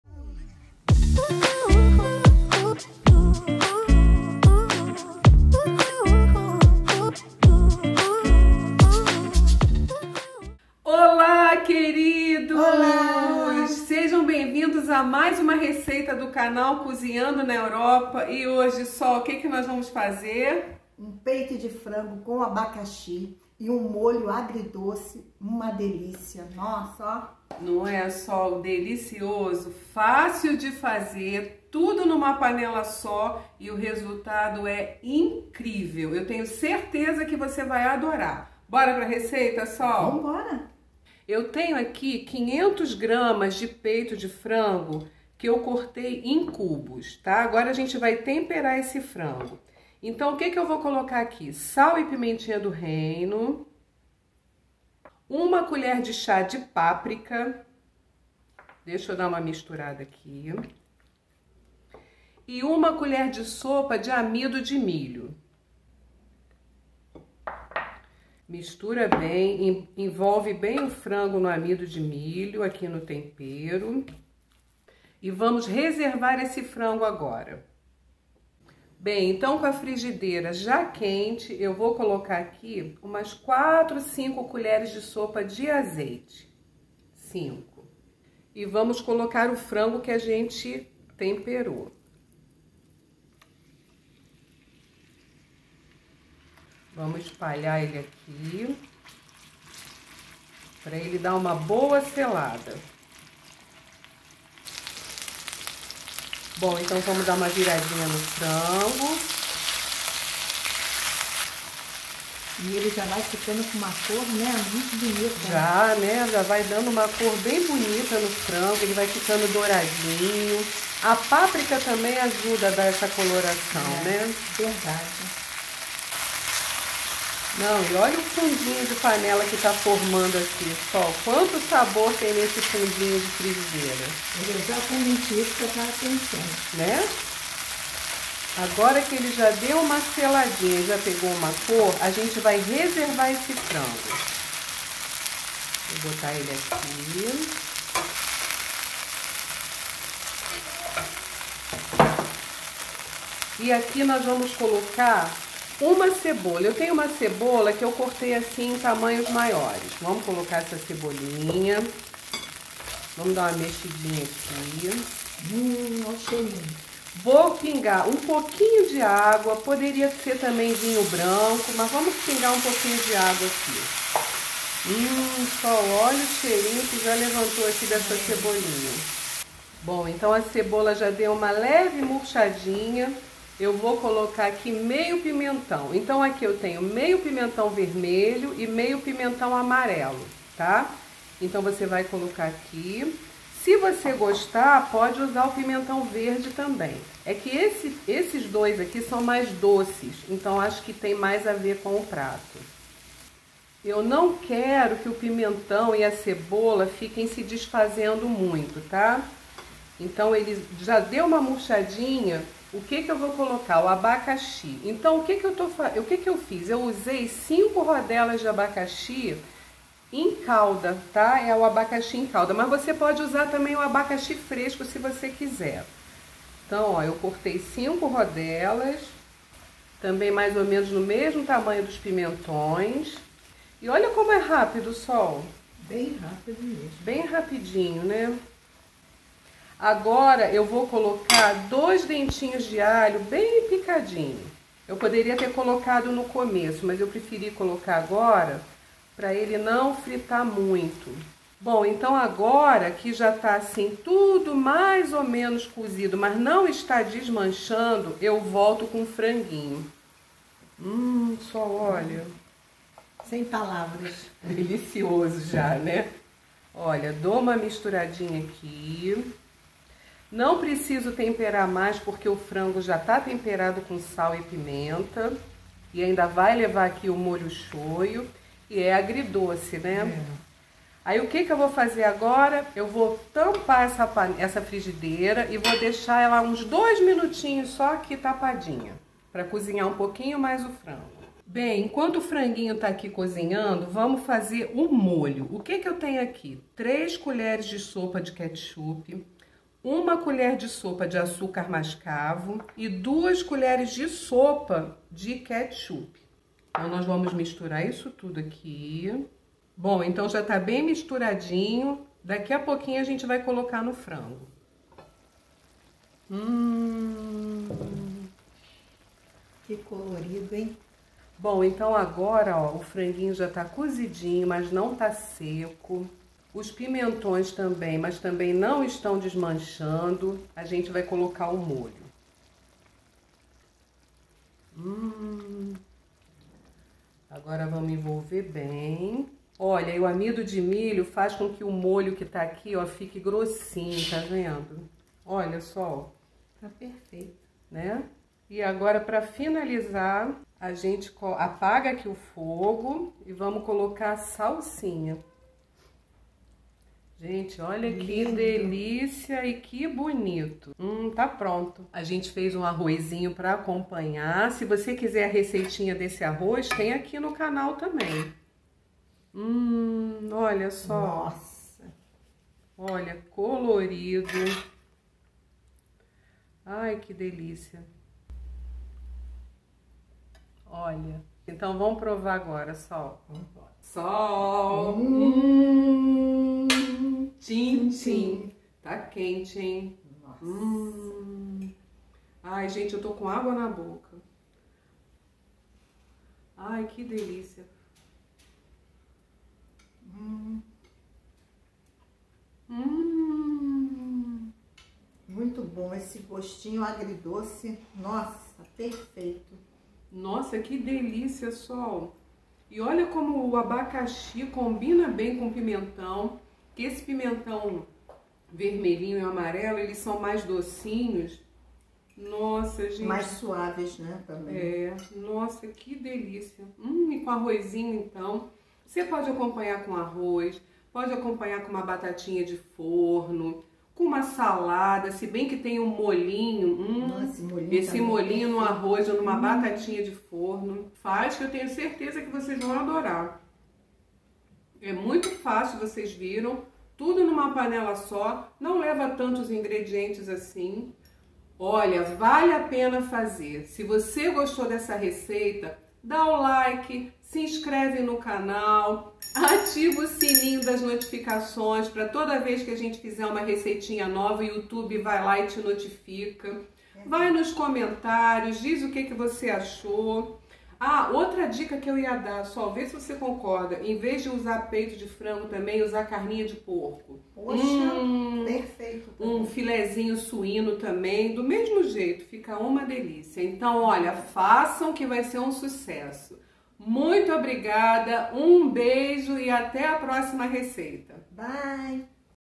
Peito de frango com abacaxi com molho agridoce
Vou te ensinar a fazer um delicioso peito de frango com abacaxi com molho agridoce que é simplesmente maravilhoso, fácil de fazer, super rápido  e tudo em uma só panela.
Tenho certeza absoluta que você vai amar esse peito de frango com abacaxi com molho agridoce.
Sai da mesmice e faz esse frango, vai seu um sucesso.

Ingredientes:

- 500g de peito de frango
- sal, pimenta-do-reino
- 1 colher de chá de páprica
- 1 colher de sopa de amido de milho
- 5 colheres de sopa de azeite
- 1 cebola
- 1/2 pimentão vermelho
- 1/2 pimentão amarelo
- 2 dentes de alho
- 5 rodelas de abacaxi em calda (pode ser o fresco)
- salsinha

Para o molho agridoce:

- 3 colheres de sopa de molho shoyu
- 2 colheres de sopa de ketchup
- 1 colher de sopa de açúcar mascavo

Modo de preparo como apresentado no vídeo

Muito obrigada, um beijo e
Até a próxima receita.